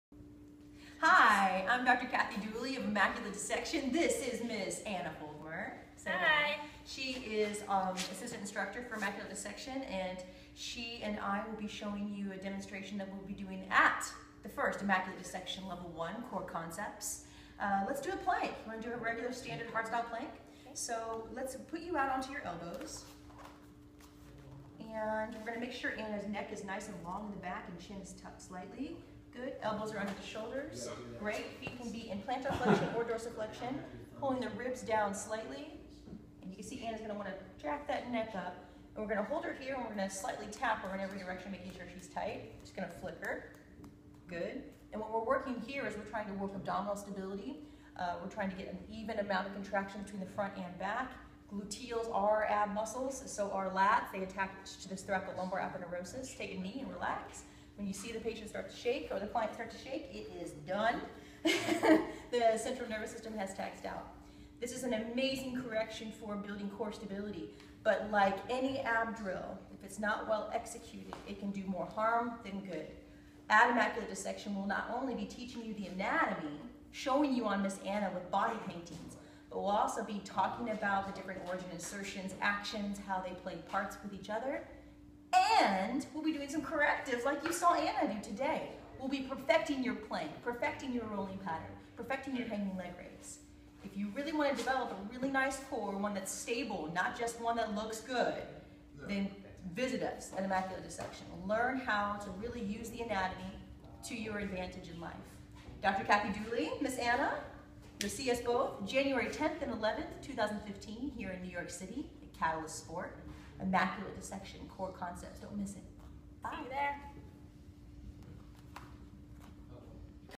DOOLEY NOTED: Dissecting The Plank - Immaculate Dissection
Dooley Noted: 12/26/2014

Immaculate Dissection: The Plank

I am thrilled about the debut of our seminar series, Immaculate Dissection.

Since many people are not ethically permitted to complete human dissection, we want to bring the laboratory to people with discussions of functional anatomy, along with guided of palpation and corrective exercise instruction.

We utilize the amazing artwork of Danny Quirk, using body painting to demonstrate the anatomy on Anna Folckomer.

Here is a corrective exercise for core musculature, involving the plank. I hope you enjoy!

- Dr. Kathy Dooley